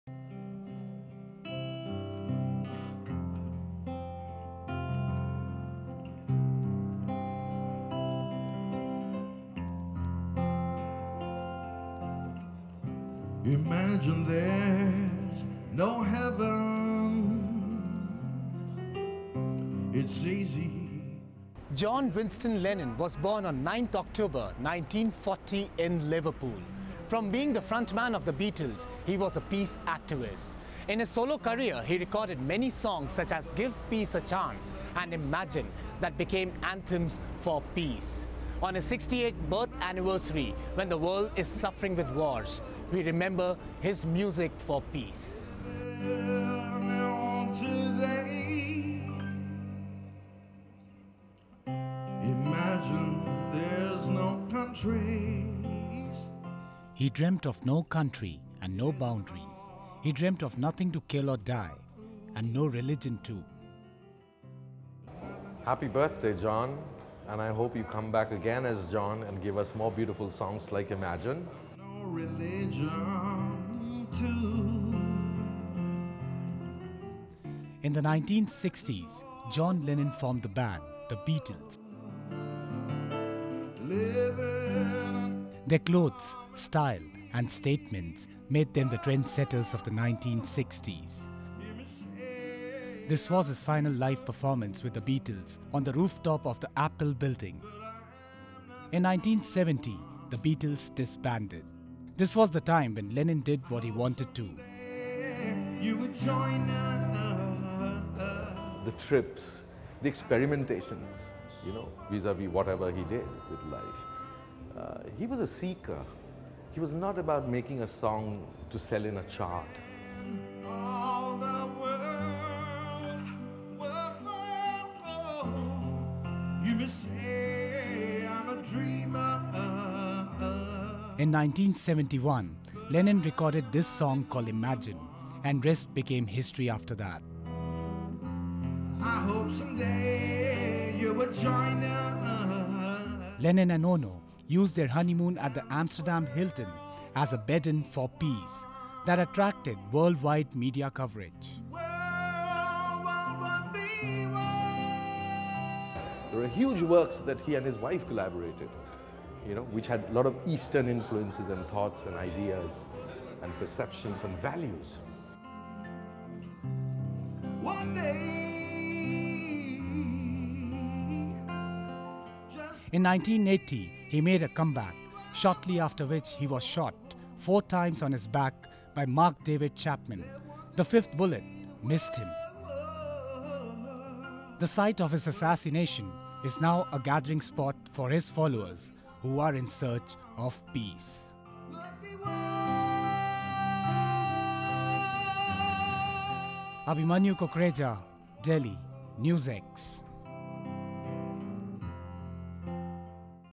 Indian tribute to John Lennon
Tribute to John Lennon aired on NewsX, a national english news channel in India